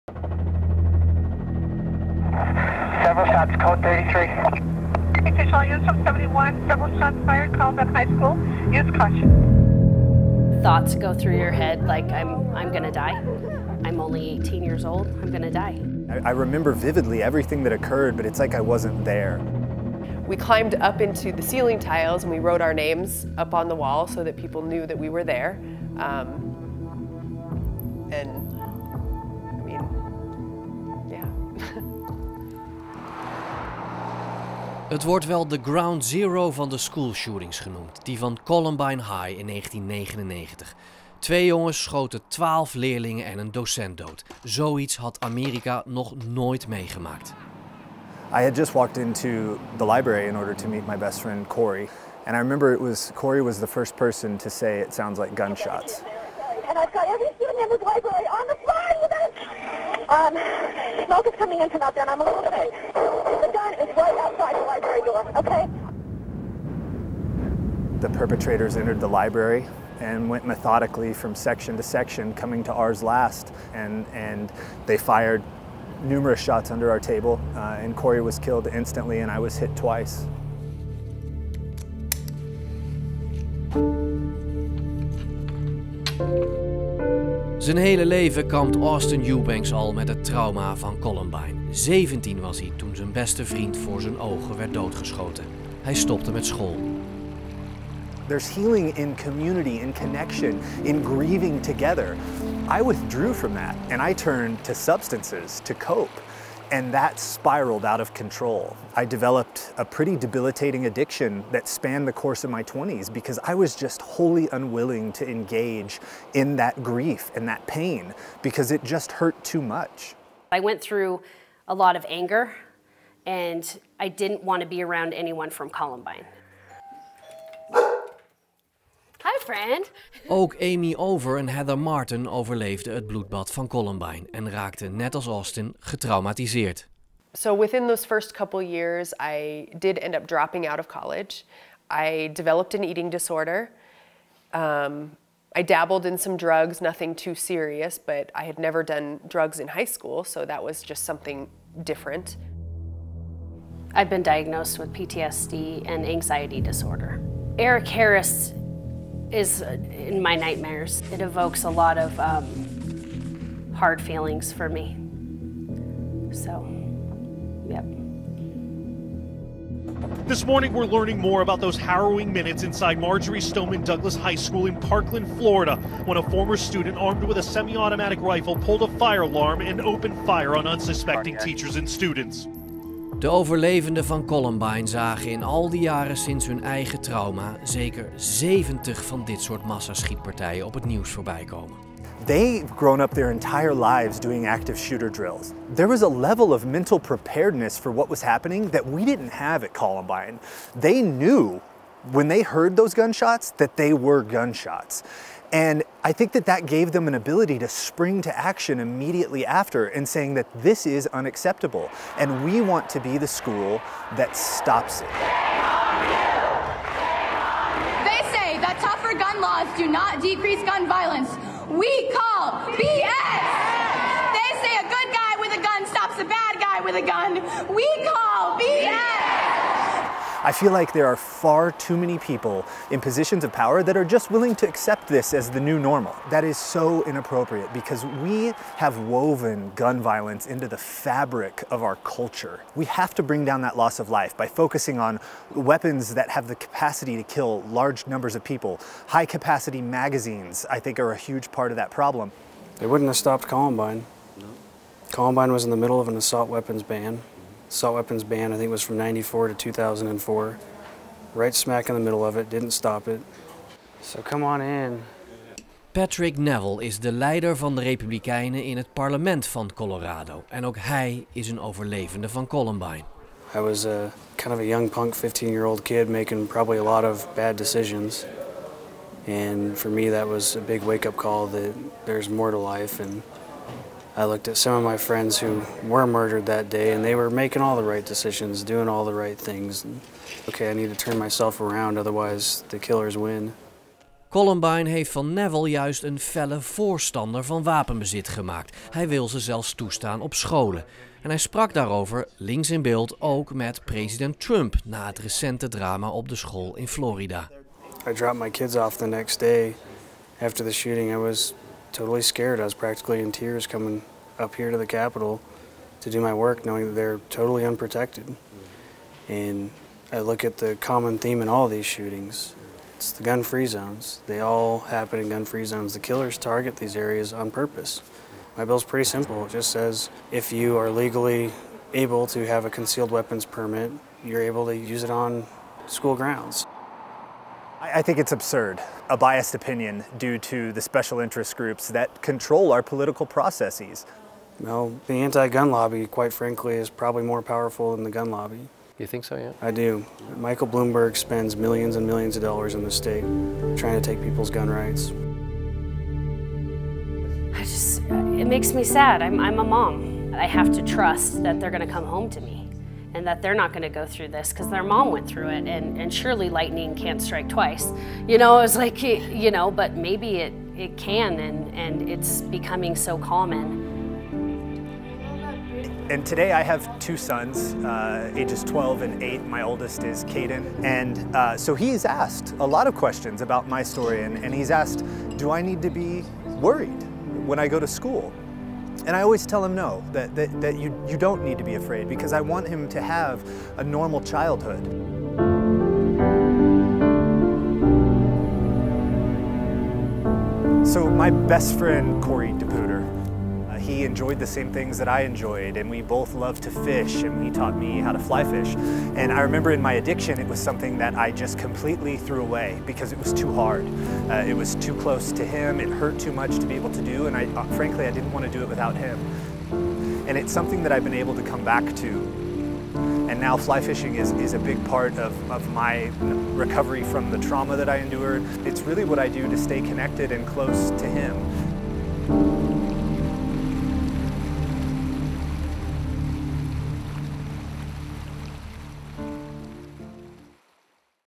columbine survivors
Columbine survivors discuss their experience in the wake of the Parkland shooting, for NOS Dutch public television, reported by Wouter Zwart